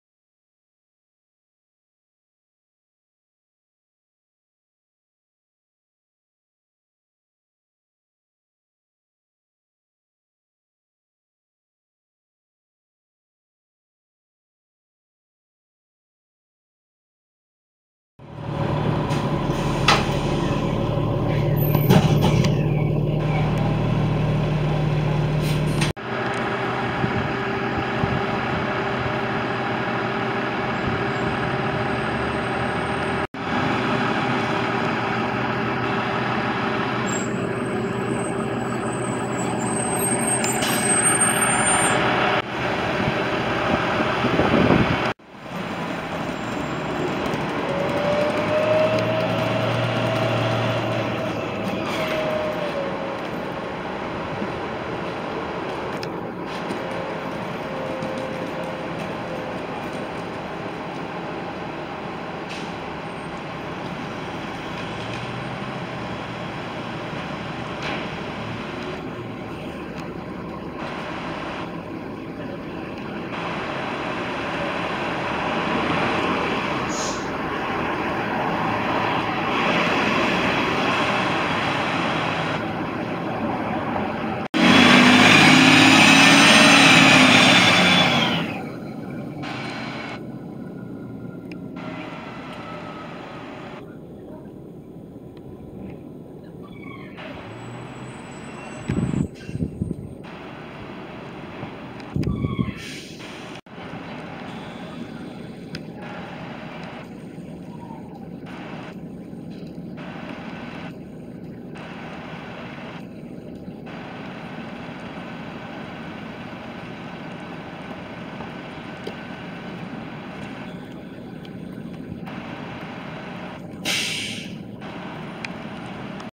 2018 Eldorado National ENC EZ Rider II 35LF
University of Delaware had 4 brand of 2018 Eldorado National ENC EZ Rider II 35 low floor, Bus # 180 and 325 are the first ones on the road and two others buses are still in not yet on the road. Those four buses replace with 2 oldest Bluebird  buses and 2 other buses.